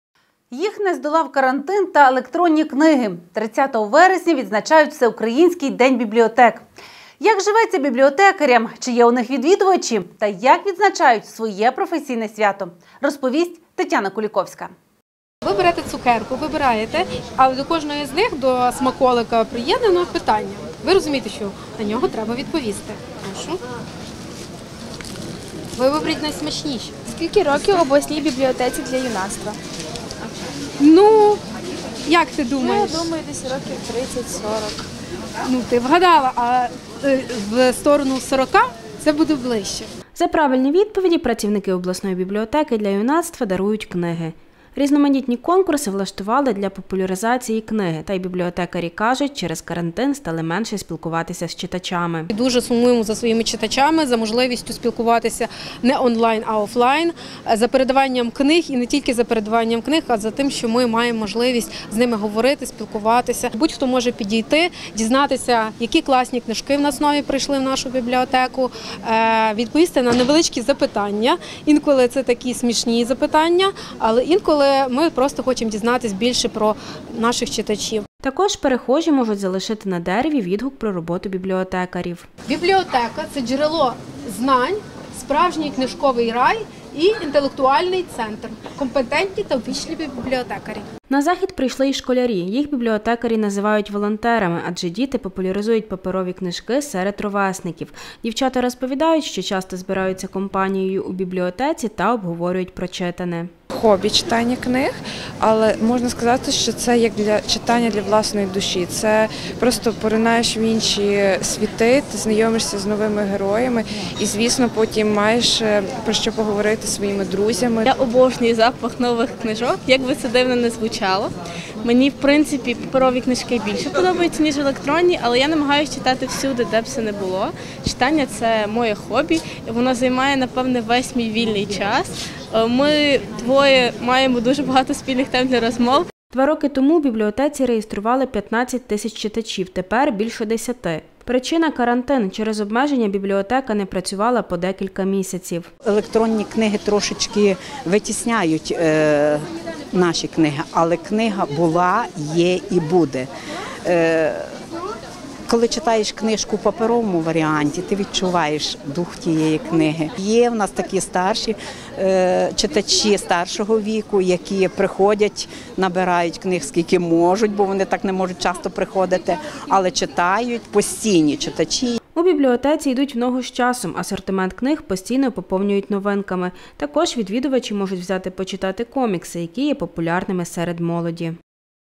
Всеукраїнський день бібліотек
Їх не здолав карантин та електронні книги. 30 вересня відзначають Всеукраїнський день бібліотек. Як живеться бібліотекарям чи є у них відвідувачі та як відзначають професійне свято?